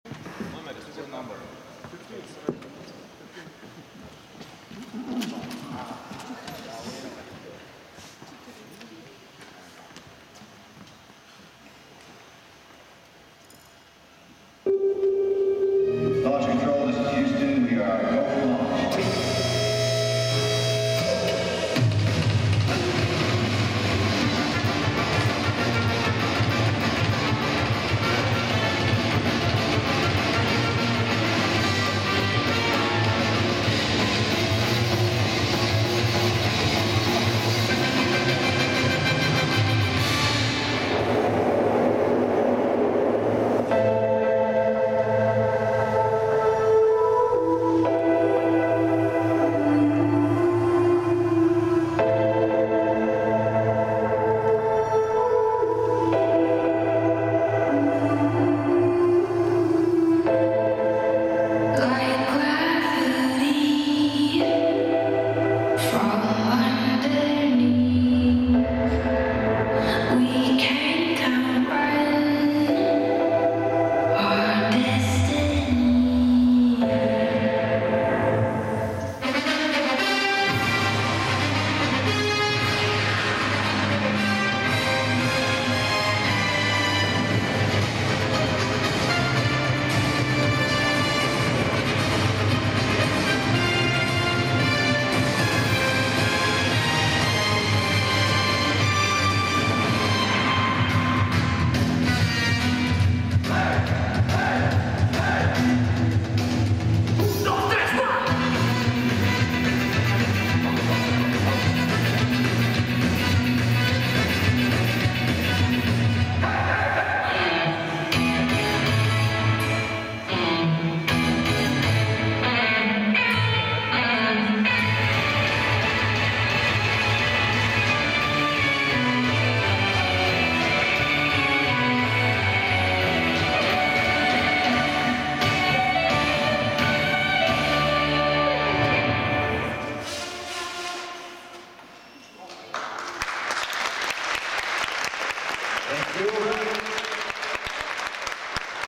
F3P World Championships 2023 - F3P-AFM - Donatas PAUZUOLIS (LTU)
Video of scored flight Round #1